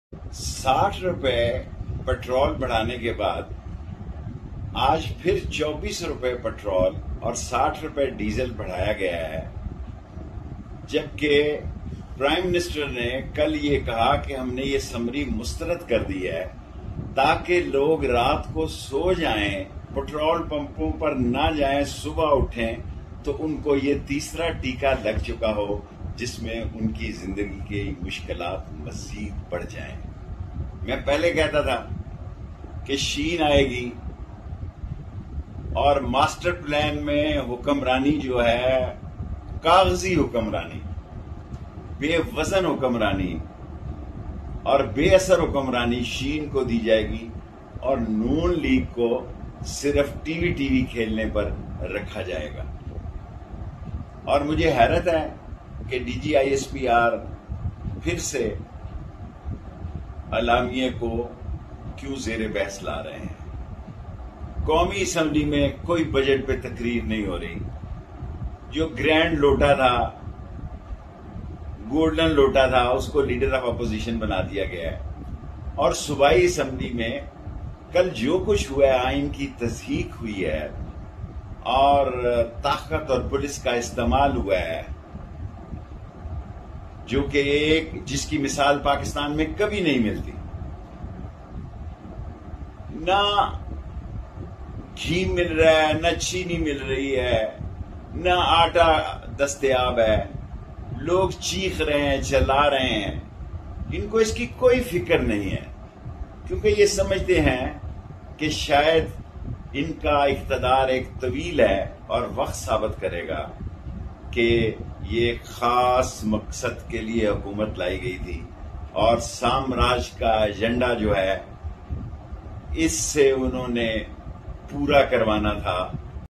Sheikh Rashid Reaction DG ISPR Babar Iftikhar Statement |شیخ رشید برس پڑے ڈی جی آئی ایس پی آر پر؟
Sheikh Rashid Reaction On DG ISPR Babar Iftikhar Statement | شیخ رشید برس پڑے ڈی جی آئی ایس پی آر پر؟